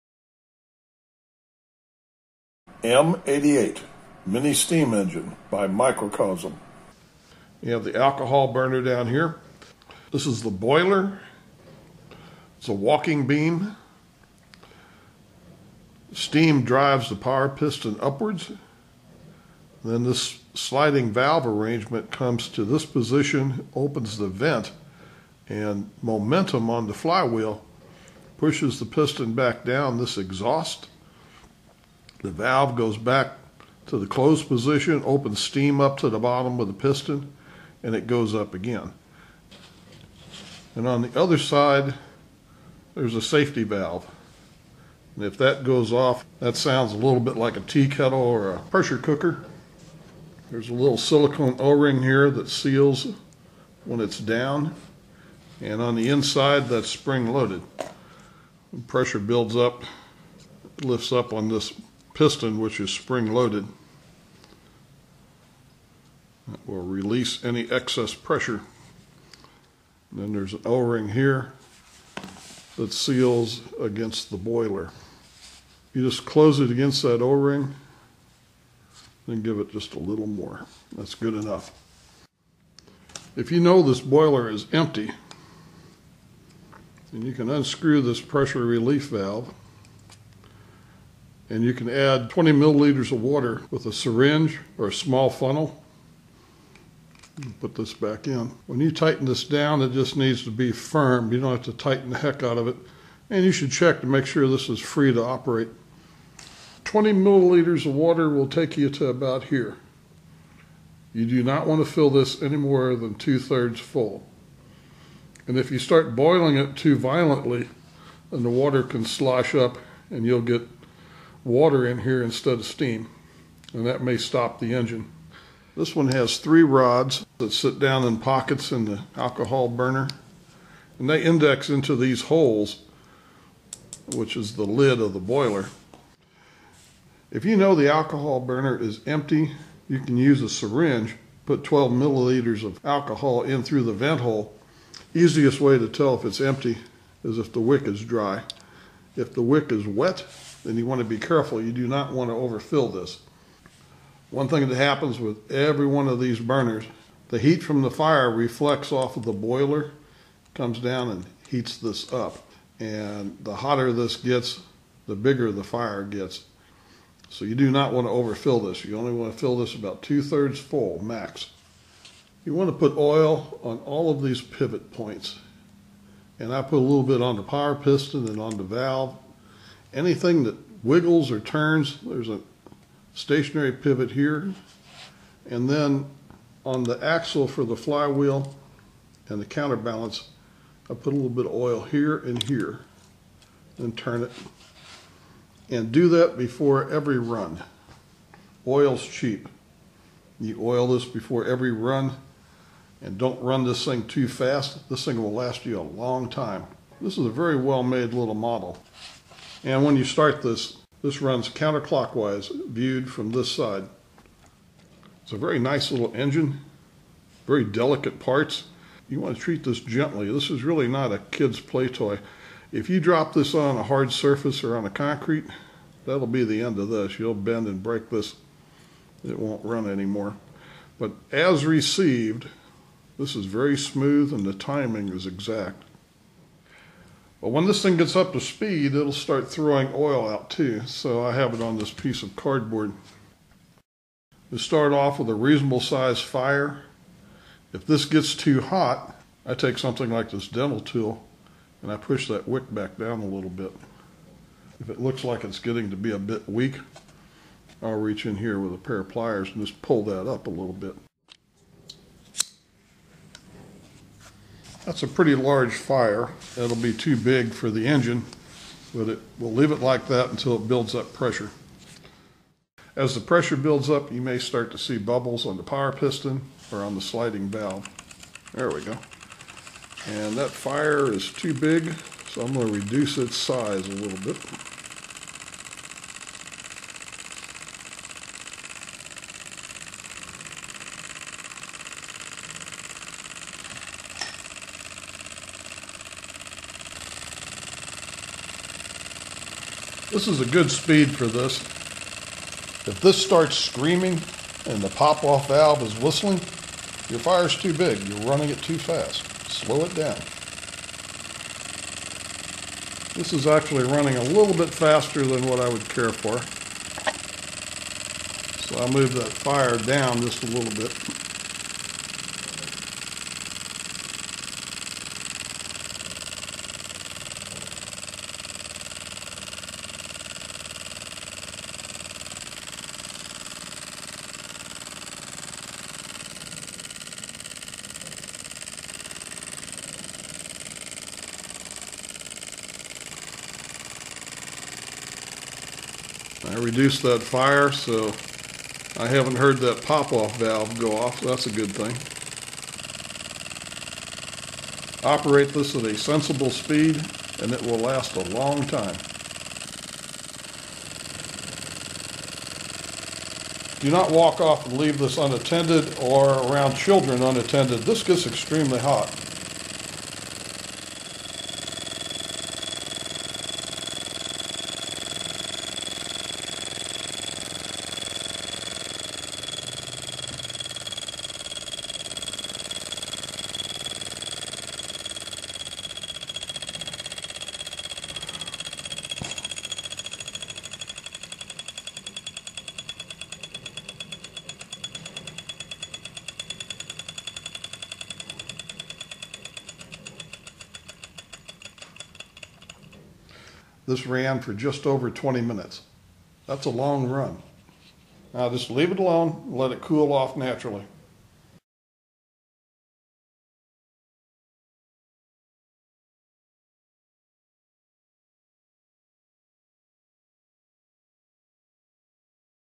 Microcosm - M88 Mini Steam Engine Model
Since Banggood is no longer interested in selling engines, get your engines here.

Re-edited video for this Fast, smooth, long-running little steam engine.